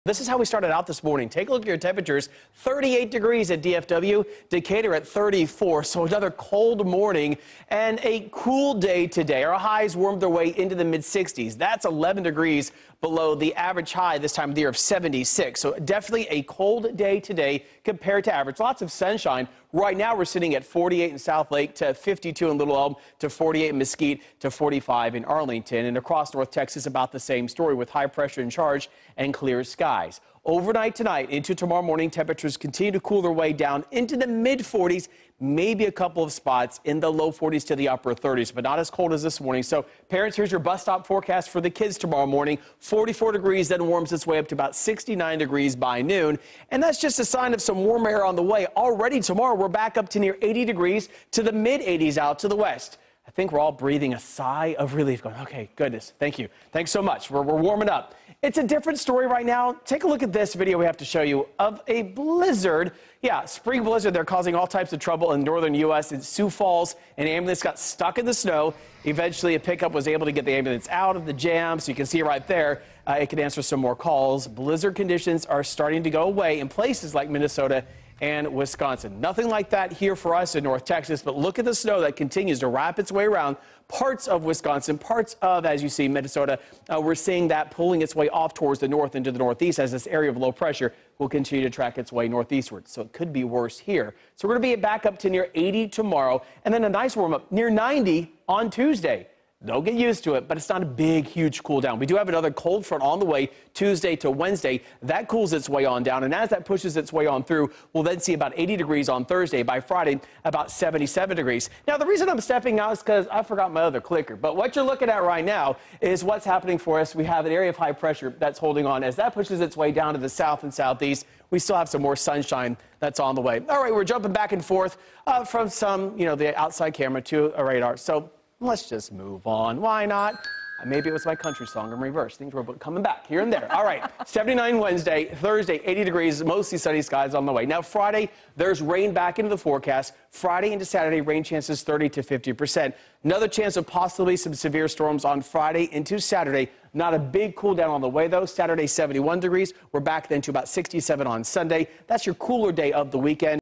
Scott Padgett's Weather Forecast
Expect a chilly Monday morning, but temperatures will warm up at the start of the week.